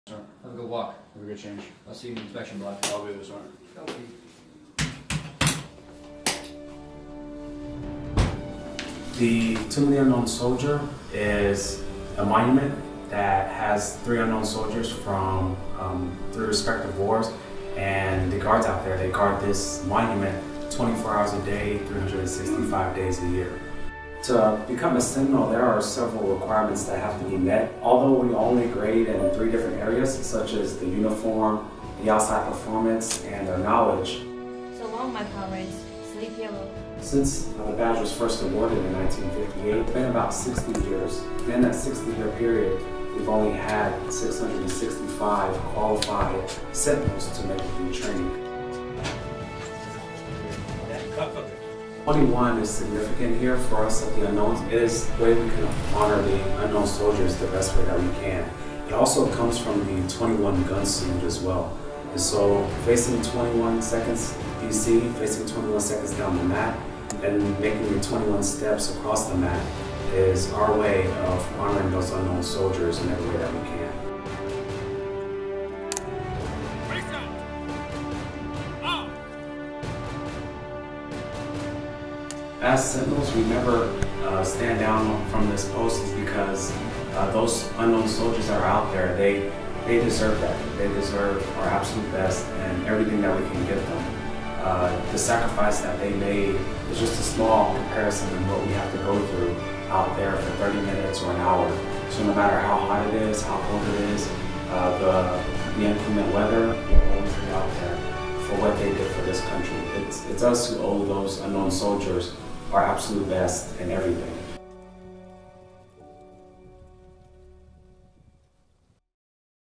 Old Guard: The Sentinels
The Old Guard never takes a day off. Here’s your chance to see what it takes to be a Tomb Guard sentinel.